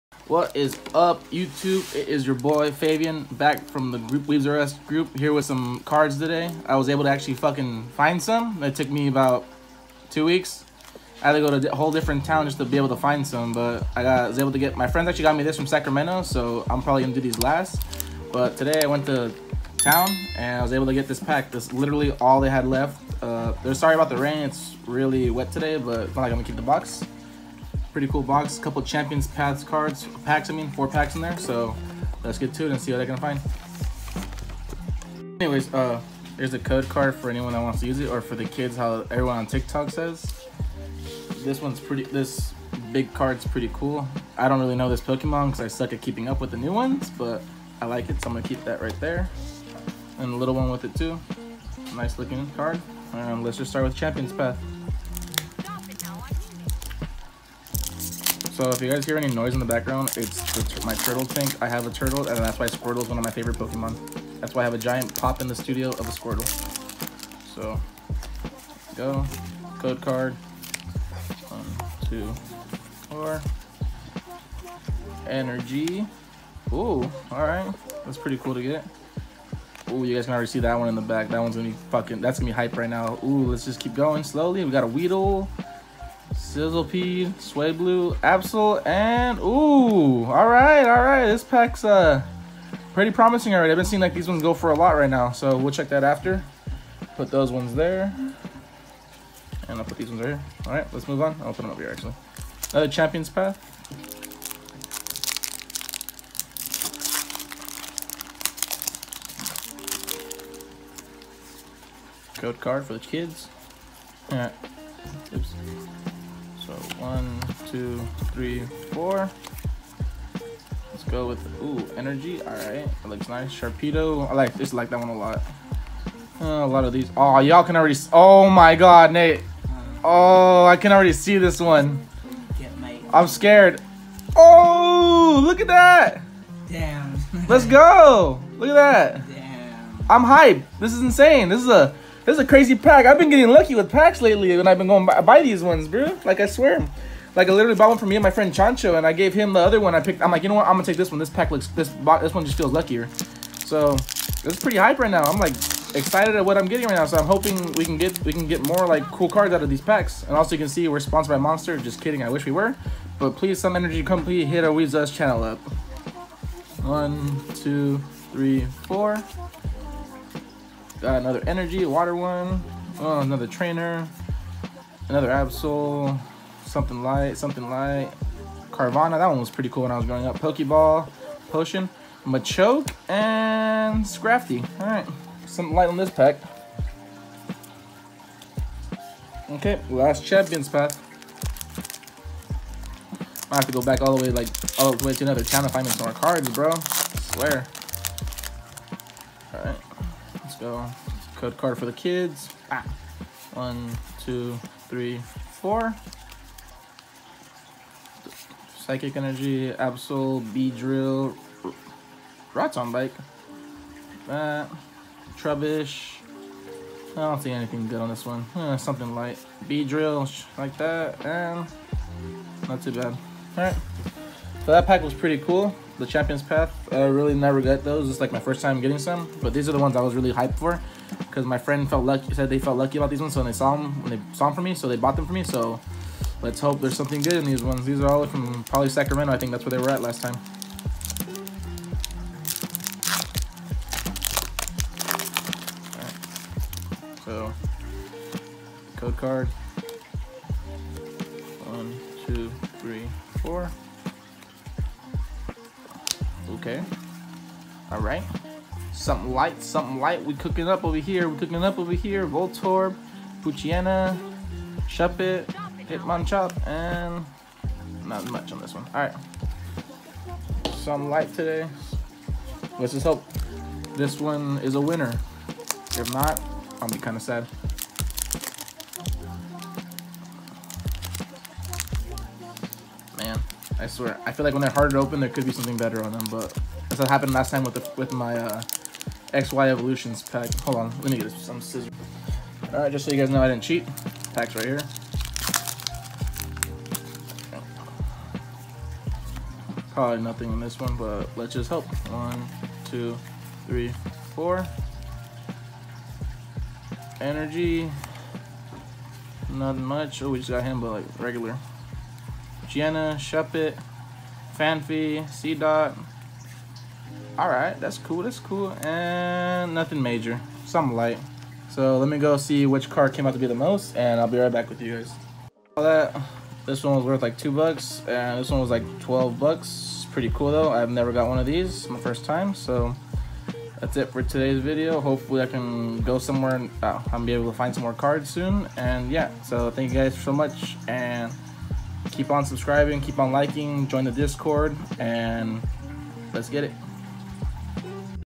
POKEMON UNBOXING #2!!! (FT FABIAN) *OMG*
This is our Pokemon unboxing series! We hope you all enjoy it and let us know who you got in your collection???